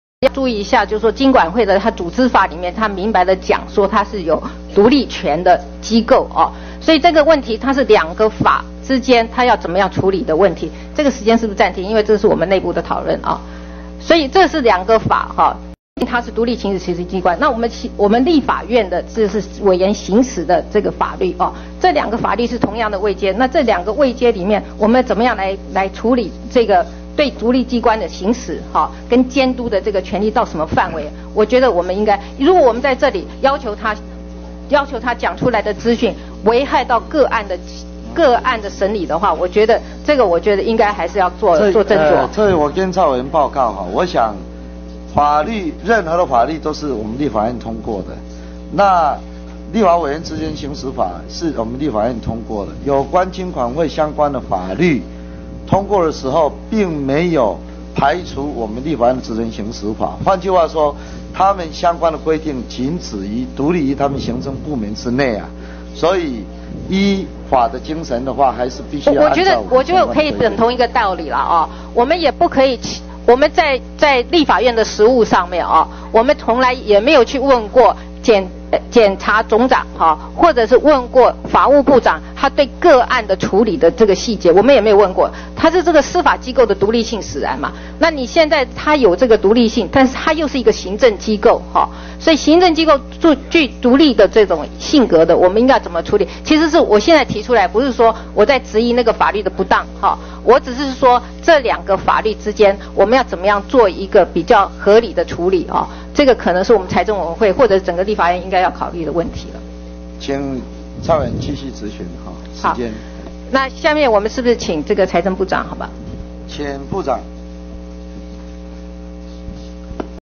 20050407 蔡英文 李全教 聯電案 約談 曹興誠 的地點 龔照勝 金管會 獨立機關 財政委員會 【立法院演哪齣？】
( 逐字稿 )主席( 李全教 )：立法院職權行使法第25條規定：「質詢之答復，不得超過質詢範圍之外。被質詢人除為避免國防、外交明顯立即之危害或依法應秘密之事項者外，不得拒絕答復。被質詢人違反第一項規定者，主席得予制止。」
蔡委員英文：可是金管會組織法明白規定他們是獨立行使職權的機構，所以是兩個法產生競合後要如何處理的問題，立法院職權行使法和金管會的組織法位階相同，這關係到對獨立機關行使監督權的範圍，如果我們要求他們所公開的資訊會危害個案的審理，本席認為還是應該要斟酌。
主席：任何法律都是由立法院所制定，當初通過金管會組織法時並沒有排除立法院職權行使法的適用，換言之，他們的相關規定僅止於他們行政部門之內，所以還是應該要按照我們的規定。
蔡委員英文：在立法院的實務上，從來沒有問過檢察總長或法務部長對個案處理的細節，因為這是司法機構的獨立性使然，現在他們有獨立性，又是一個行政機構，到底應該要怎麼處理？本席並不是在質疑那個法律的不當，只是認為這兩個法律之間應該要作比較合理的處理，這是我們財政委員會和整個立法院應該要考慮的問題。
vod 58687 sd c06 y2005 m04 d07